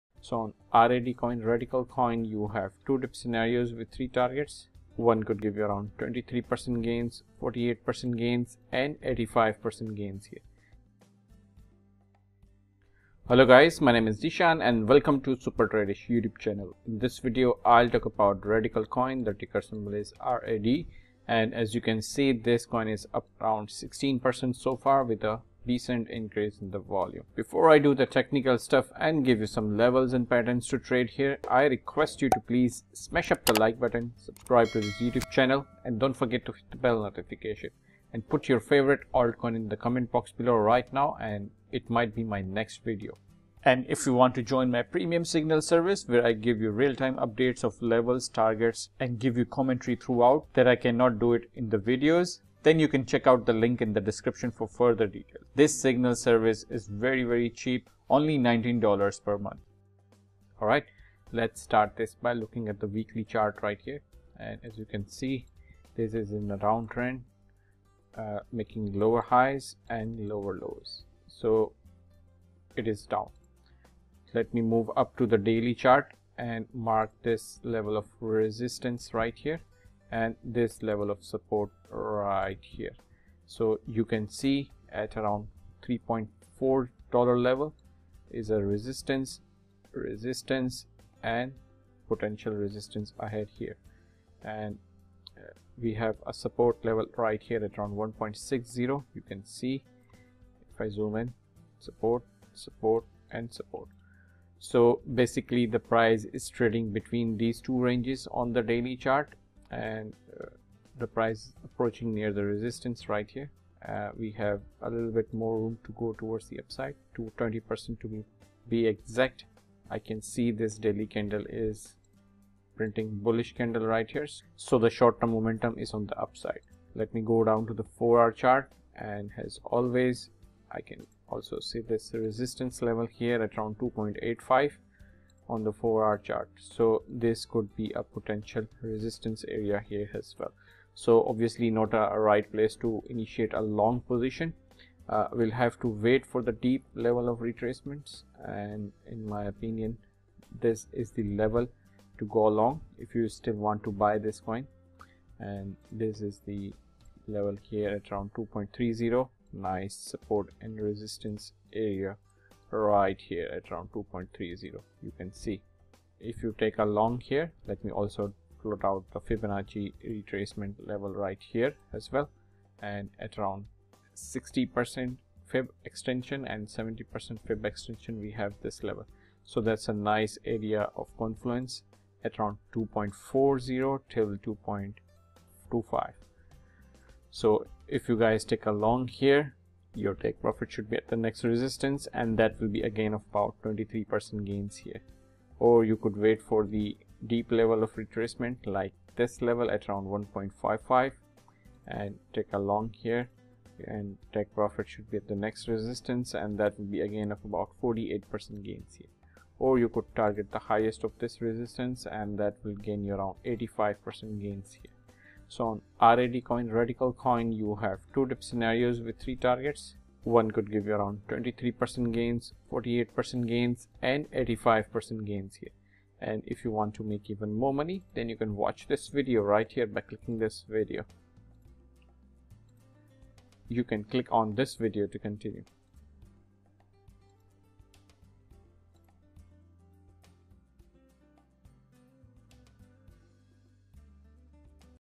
RAD Radicle Coin Price Prediction Urgent Alert (UPDATE)
This video is about RAD Radicle Coin Price Prediction in which I gave 2 dip scenarios to trade on Radicle Coin

💰⚡BYBIT $4500 DEPOSIT BONUS⚡💰TRADE RAD Radicle Coin HERE TO CLAIM THE BONUS